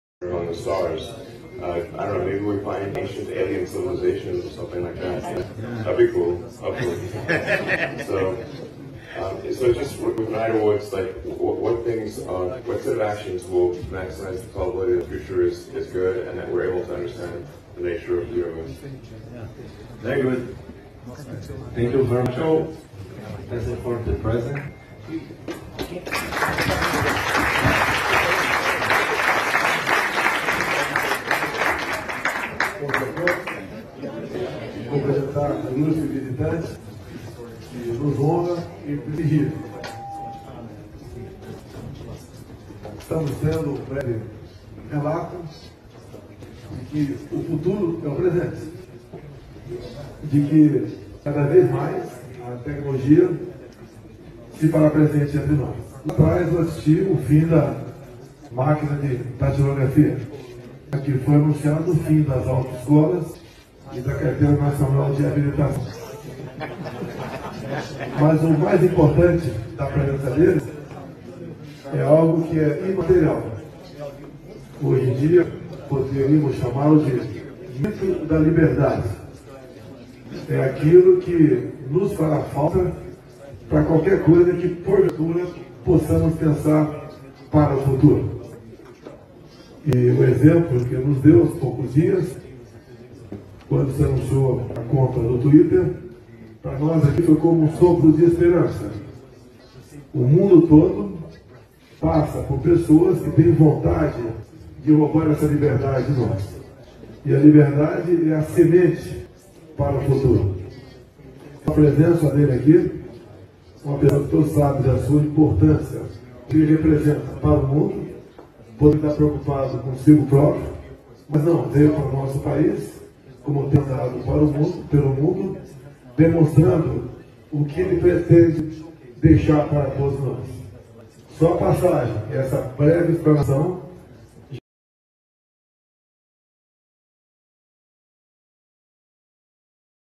Elon Musk visits Brazil and has a meeting with President Bolsonaro - Material Fact
Elon Musk visita Brasil e tem encontro com presidente Bolsonaro